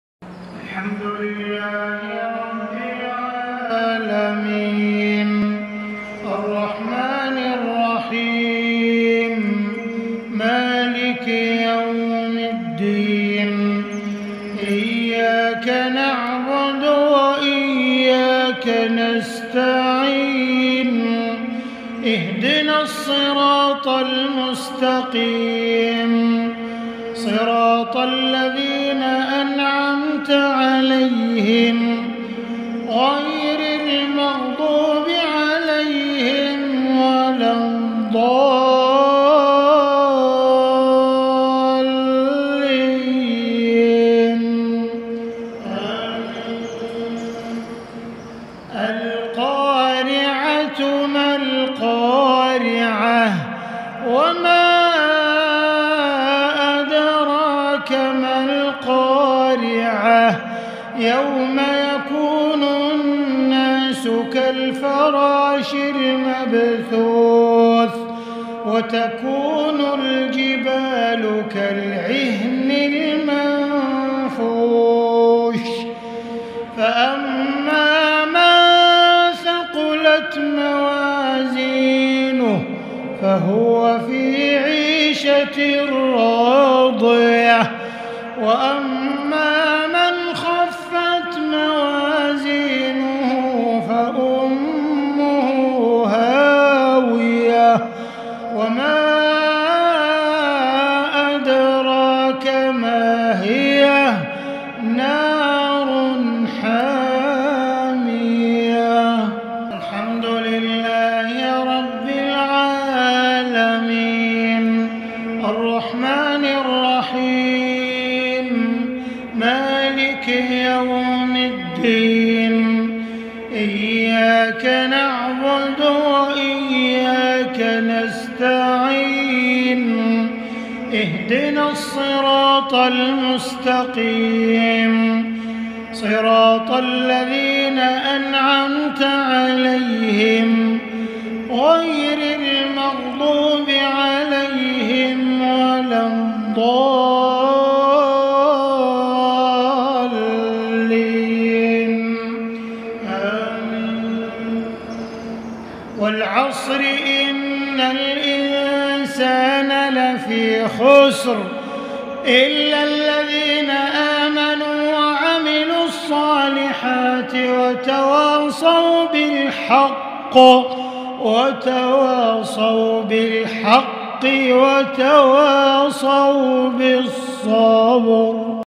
Recitation of Sheikh Abdul Rahman Al-Sudais 20 April 2021 | Maghrib Prayer | 8 Ramadan 1442h
Watch New Video Recitation of Sheikh Abdul Rahman Al-Sudais 20 April 2021 | Maghrib Prayer | 8 Ramadan 1442h PAKURBAN